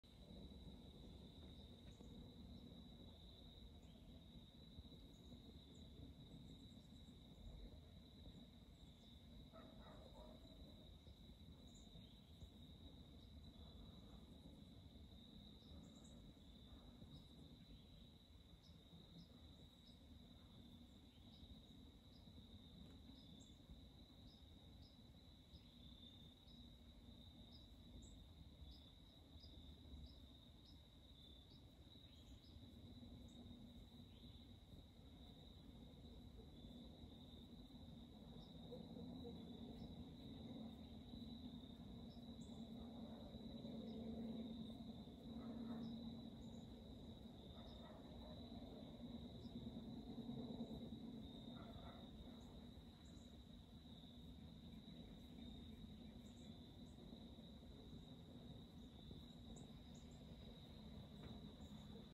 Smoky Mountains Autumn
Friday November 1st 2024
Smoky Mountains Tennessee

Fall changing of the leaves in the Smokies.